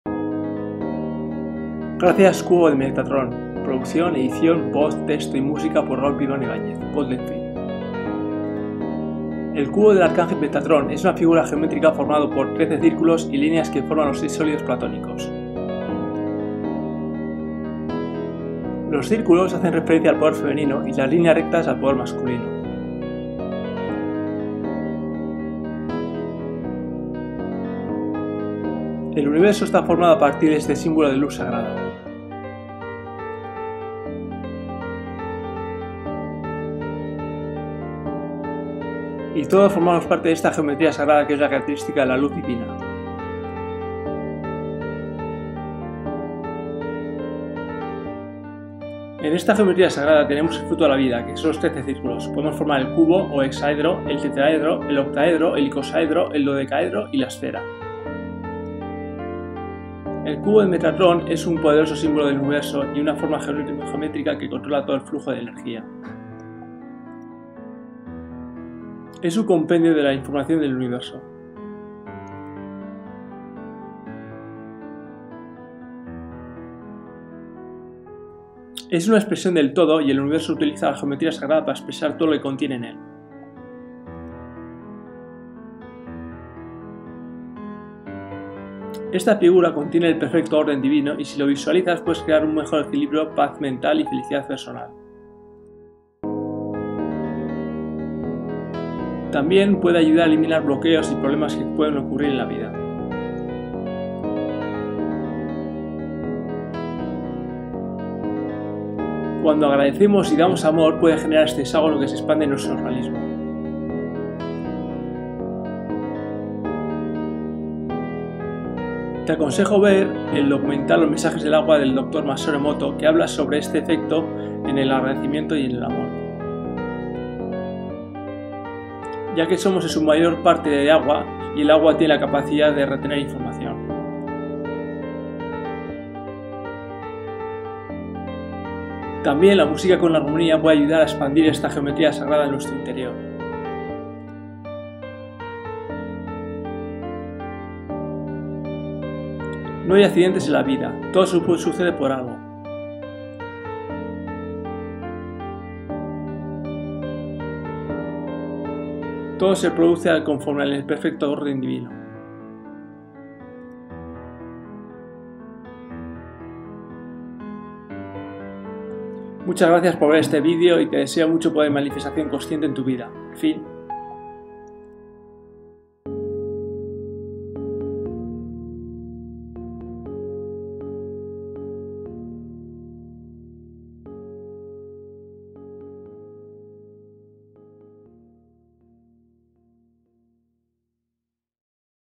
GRACIAS CUBO DE METATRON #43 .- raúl pipaón ibáñez - goldenphi
Aqui el texto en castellano:

El cubo del arcangel Metatron es una figura geometrica formado por 13 circulos y lineas que forman los 6 solidos platónicos. Los circulos hacen referencia al poder femenino y las lineas rectas al poder masculino. El universo esta formado a partir de este simbolo de luz sagrada. Y todos formamos parte de esta geometria sagrada que es la caracteristica de la luz divina. En esta geometria sagrada tenemos el fruto de la vida que son los 13 circulos, podemos formar el cubo o hexaedro, el tetrahedro, el octaedro, el icosaedro, el dodecaedro y la esfera. El cubo de Metatron es un poderoso simbolo del universo y una forma geometrica que controla todo el flujo de energia. Es un compendio de la información del universo. Es una expresion del todo y el universo utiliza la geometria sagrada para expresar todo lo que contiene en él. Esta figura contiene el perfecto orden divino y si lo visualizas puedes crear un mejor equilibrio, paz mental y felicidad personal. También puede ayudar a eliminar bloqueos y problemas que pueden ocurrir en tu vida. 11 Cuando agradecemos y damos amor puede generar este hexagono del hexaedro que se expande en nuestro organismo. Te aconsejo que veas el documental los mensajes del agua del doctor Masaru Emoto que habla sobre sobre el efecto del agradecimiento y el amor. Ya que somos en su mayor parte agua y el agua tiene la capacidad de retener información. También la musica con armonia puede ayudar a expandir esta geometria sagrada en nuestro interior. No hay accidentes en la vida, todo sucede por algo. Todo se produce conforme al perfecto orden divino. FIN.